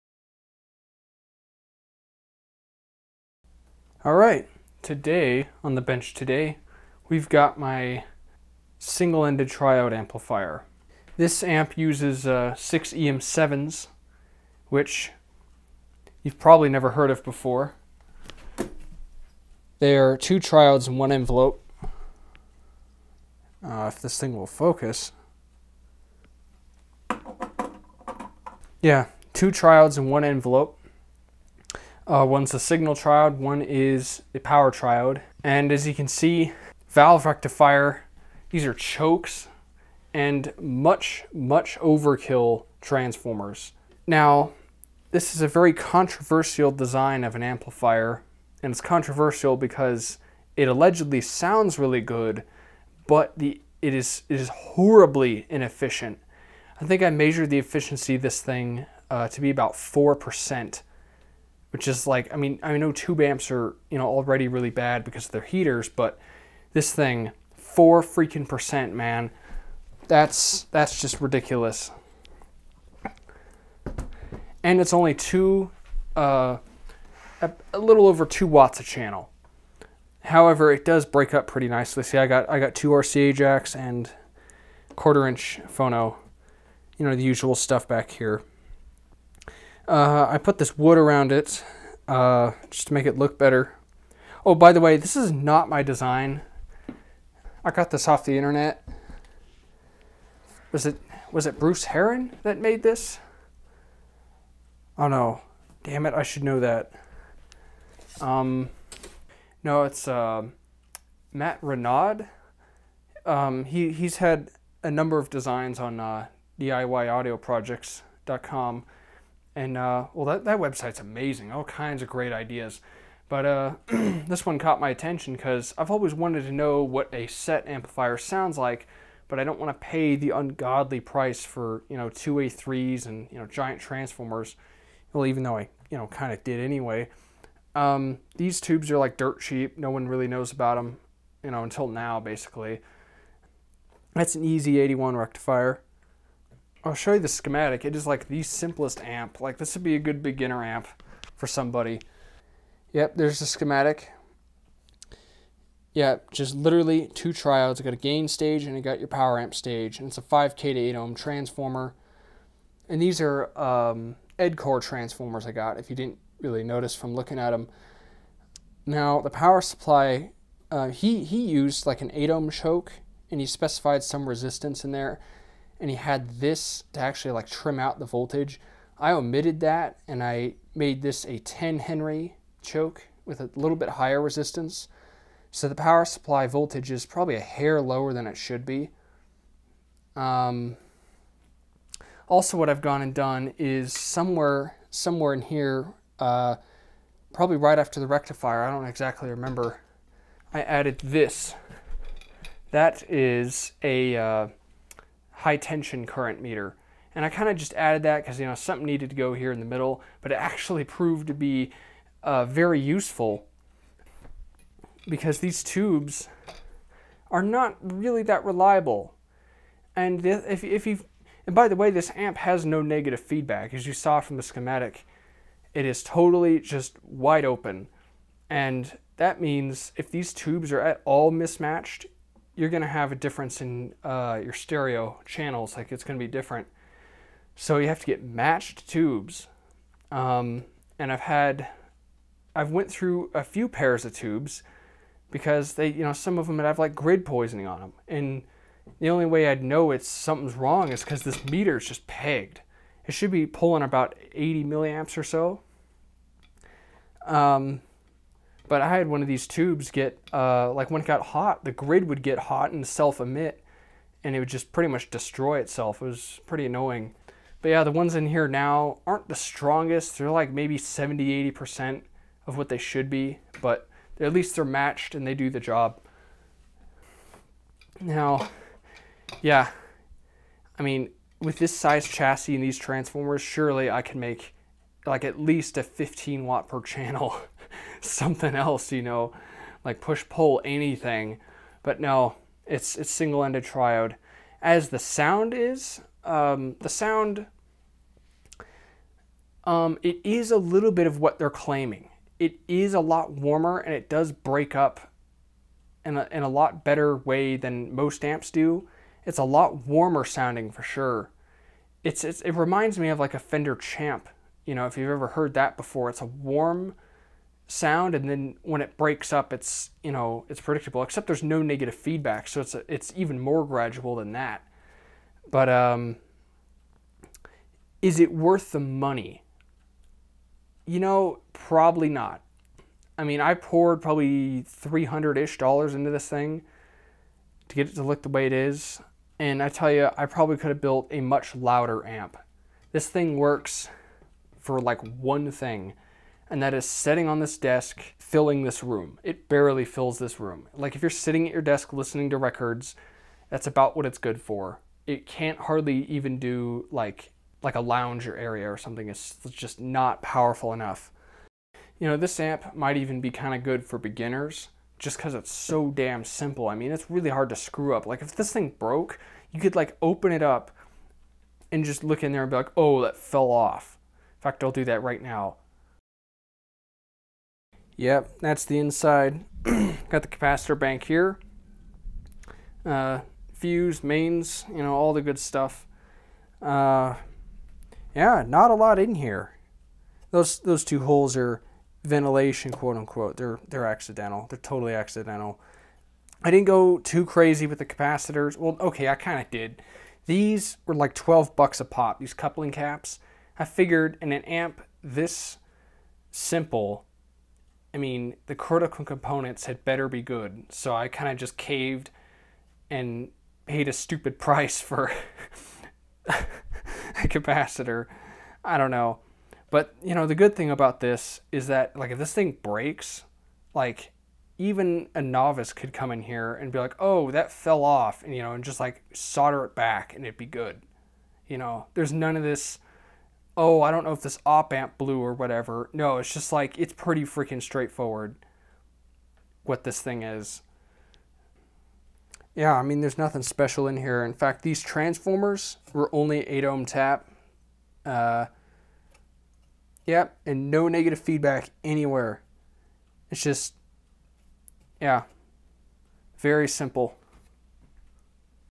6EM7 SET Amplifier
This is my 6EM7 Single-Ended Triode Hi-Fi amp that sits on my desk. I use it almost every day and it sounds great. It's only 2.2 watts per channel (probably less with these tubes) but fills the small room well enough. There are no solid-state components in this amp, it's all wood, glass and steel. Solid-state fanboys click away now. I did not design this circuit, however I did design the enclosure. It uses Edcor transformers which are way overrated for the current being drawn, perhaps to give better low and high-end frequency response.

Power efficiency is actually something like 8%.